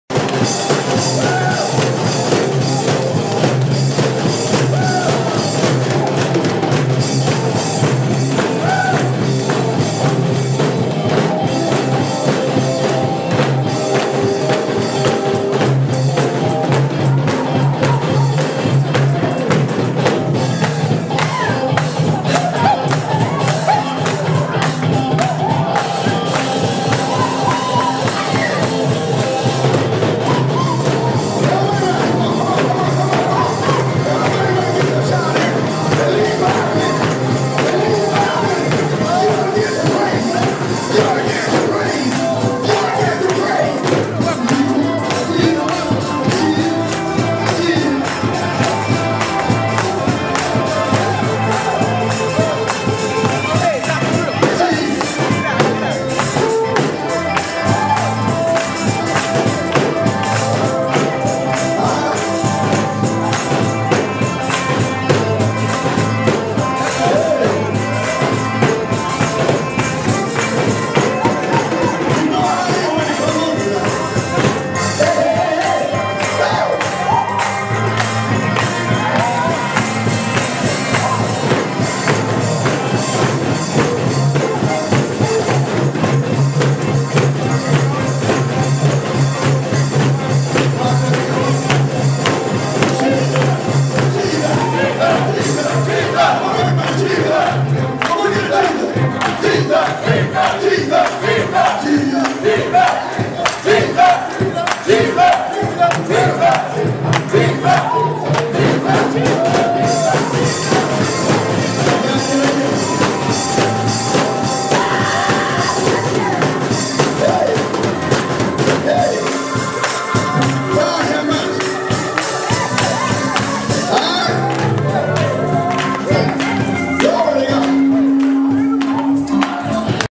PRAISE BREAK 2
THIS IS A CLIP OF ONE OF MANY PRAISE BREAKS THAT OCCURED DURING OUR EVANGELIST QUARTERLY MEETING IN SANFORD, FL AT ST JAMES HOUSE OF PRAYER